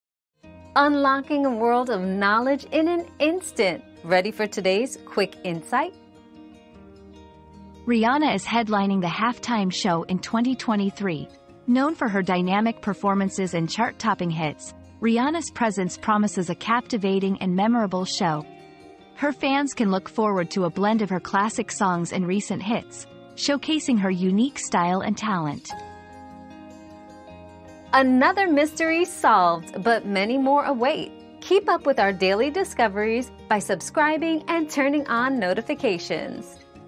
Who is doing the halftime show in 2023?
Rihanna to Headline Halftime Show in 2023 👉 Rihanna's Halftime Show 👉 Get ready for an unforgettable halftime show in 2023 as superstar Rihanna takes the stage! Known for her chart-topping hits and electrifying performances, Rihanna promises to deliver a show that will leave you wanting more. Don't miss out on this iconic moment as she showcases her unique style and talent with a blend of classic songs and recent hits!

Laura S. Harris (2024, February 2.) Who is doing the halftime show in 2023?
    🌐 AskAbout.video